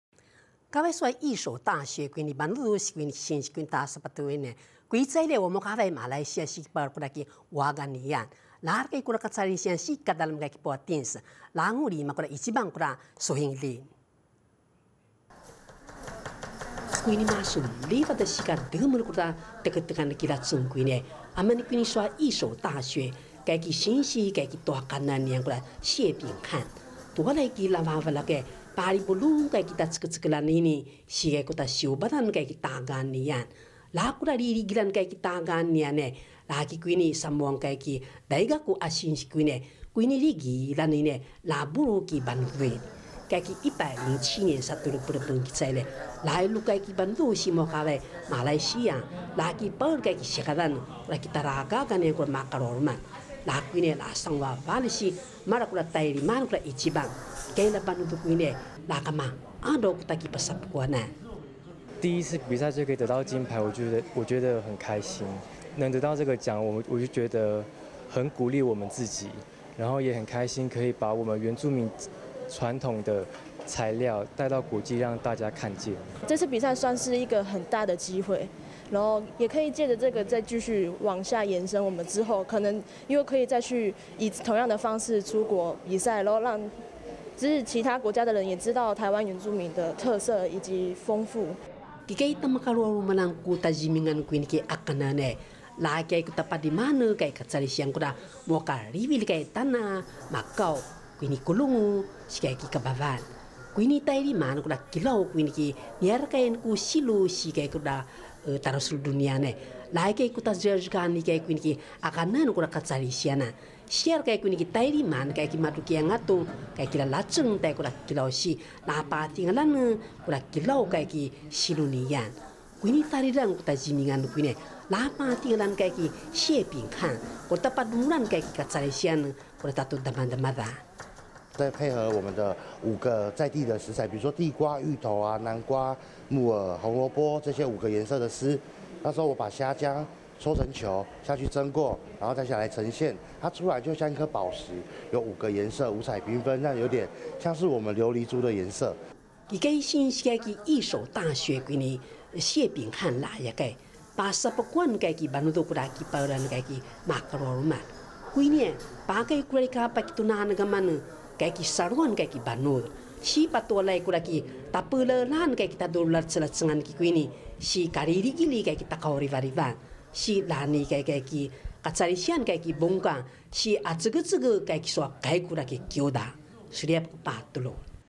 世界名廚賽義守奪5金 指導老師示範獲獎料理 2019-03-15 Rukai IPCF-TITV 原文會 原視族語新聞
kay Isutasi ngua taylrimane ku icibang ki twadamay

快速又精準的切菜手法，他是義守大學觀光餐旅學院教師謝秉翰，從小協助家中餐廳經營，廚藝經驗豐富，踏入大學教職，把一身廚藝功夫教授學生，107年底帶學生前往馬來西亞參加世界名廚大賽，一口氣拿下5面金牌，學生受到很大鼓舞。

這次比賽所設計的餐點分別融入原住民特有的刺蔥、馬告、芝麻葉、小米酒等元素，其中「五彩琉璃蝦」刻意在食材雕上原民圖騰，並用五色蔬果呈現琉璃色澤，精巧的設計，展現謝秉翰推廣原民文化的做菜初衷。

義守大學原住民族學院老師謝秉翰表示鼓勵學生參與競賽，主要是為了培養學生自信，而透過介紹菜餚，訓練口說能力，同時讓原住民文化站上國際舞台。